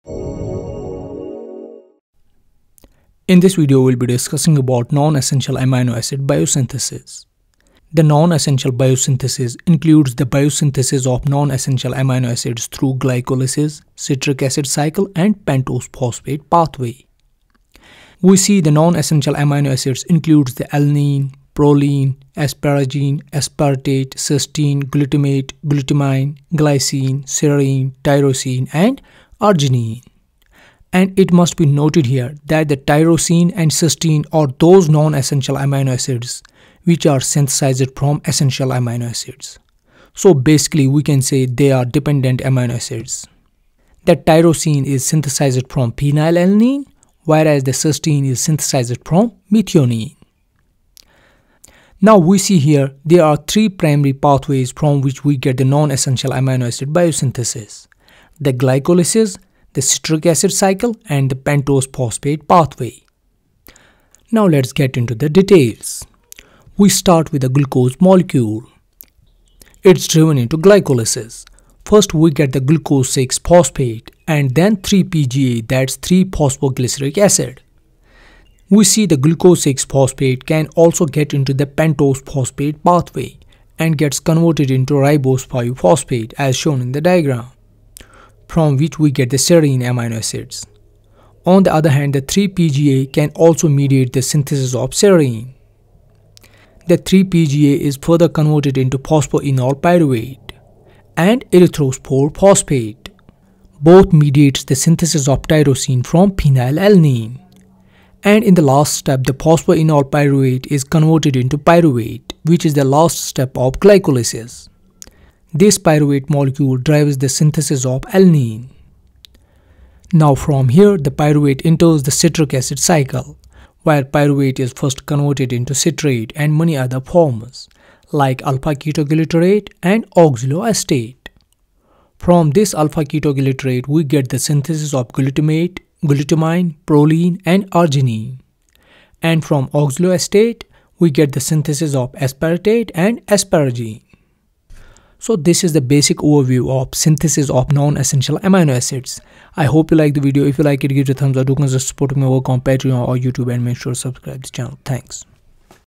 Biosynthesis of Non Essential Amino Acids | Basic Overview
Six other amino acids are considered conditionally essential in the human diet, meaning their synthesis can be limited under special pathophysiological conditions, such as prematurity in the infant or individuals in severe catabolic distress These six are arginine, cysteine, glycine, glutamine, proline, and tyrosine. Six amino acids are non-essential (dispensable) in humans, meaning they can be synthesized in sufficient quantities in the body. These six are alanine, aspartic acid, asparagine, glutamic acid, serine,and selenocysteine (considered the 21st amino acid). Pyrrolysine (considered the 22nd amino acid),which is proteinogenic only in certain microorganisms, is not used by and therefore non-essential for most organisms, including humans.

The limiting amino acid is the essential amino acid which is furthest from meeting nutritional requirements.This concept is important when determining the selection, number, and amount of foods to consume because even when total protein and all other essential amino acids are satisfied if the limiting amino acid is not satisfied then the meal is considered to be nutritionally limited by that amino acid.